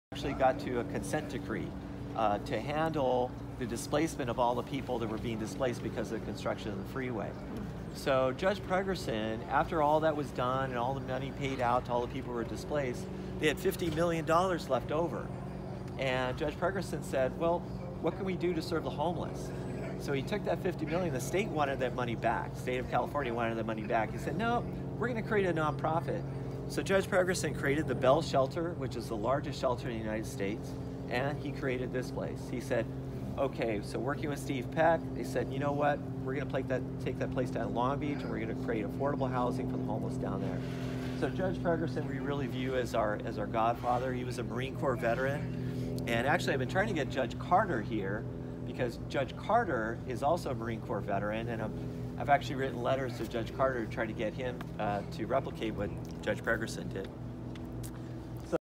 Judge Pregerson, Godfather of Century Villages@Cabrillo, Long Beach
Rene M. Castro, MSW, Director, Community Engagement, CVC briefly recounts the story of the role played by the federal Judge Pregerson in the founding of The Villages@Cabrillo, Long Beach to the Homelessness Demystified group on tour 03/15/2019.  We are standing in front of the mural which depicts many of the important places, people, and values of Century Villages@Cabrillo (CVC).

This was a Housing Experience in the Homelessness Demystified: Buffet of Educational Experiences, Feb. 2 - May 4, 2019, offering ~25 experiences in 7 different categories.
Type of Housing: Integrated Housing modeling Housing First
Service Provider, Property Developer and Manager: Century Villages at Cabrillo, Long Beach

To learn about Homelessness Demystified and participate in the Buffet of Educational Experiences - Feb.